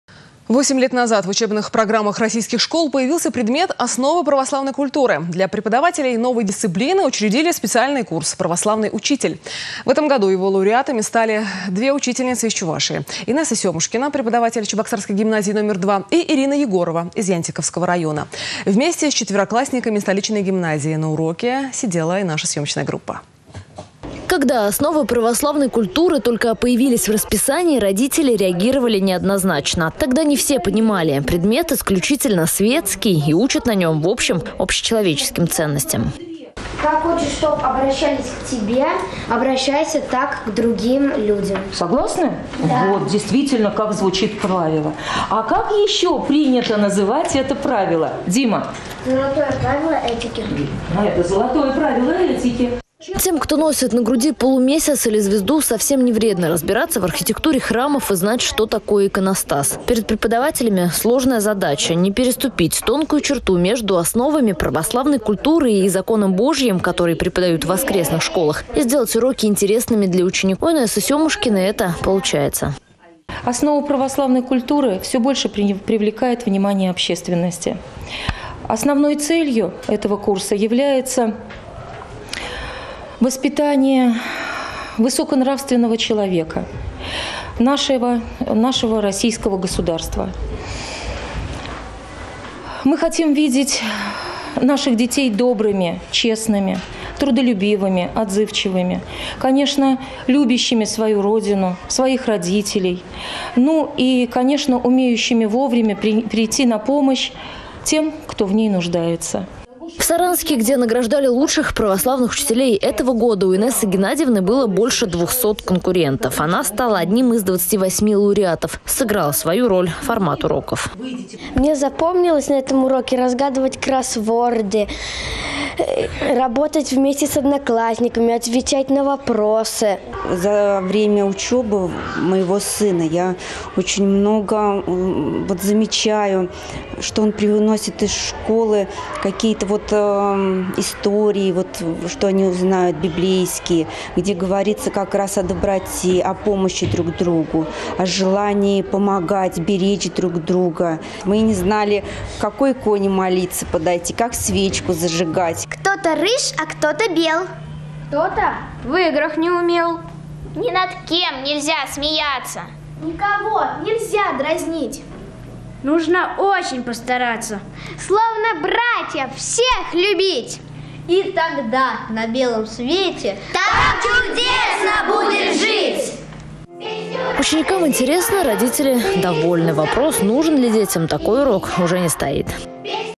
«Православный учитель 2015»
8 лет назад в учебных программах российских школ появился предмет «Основы православной культуры». Для преподавателей новой дисциплины учредили специальный конкурс «Православный учитель». В этом году его лауреатами стали 2 учительницы из Чувашии: Инесса  Семушкина — преподаватель Чебоксарской гимназии №2 и Ирина Егорова из Янтиковского района. Вместе с четвероклассниками  столичной гимназии на уроке сидела и наша съемочная группа.